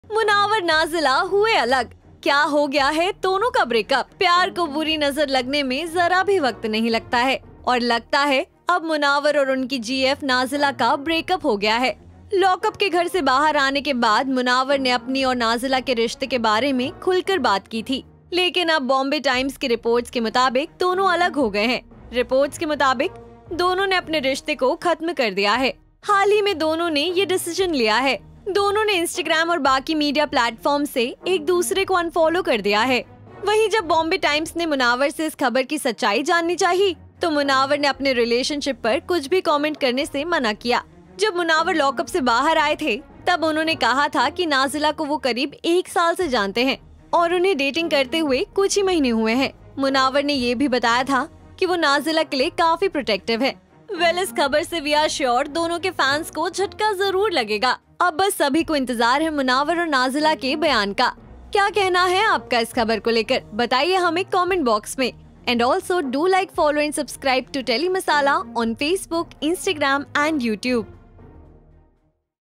Shocking ! Munawar Faruqui & Nazila Break Up ? Stand-Up Comedian Reacts
As per reports Munawar and Nazila have split-up. They both have unfollowed each other on Instagram. When a portal asked Munawar about the same he denied giving any reaction on his relationship status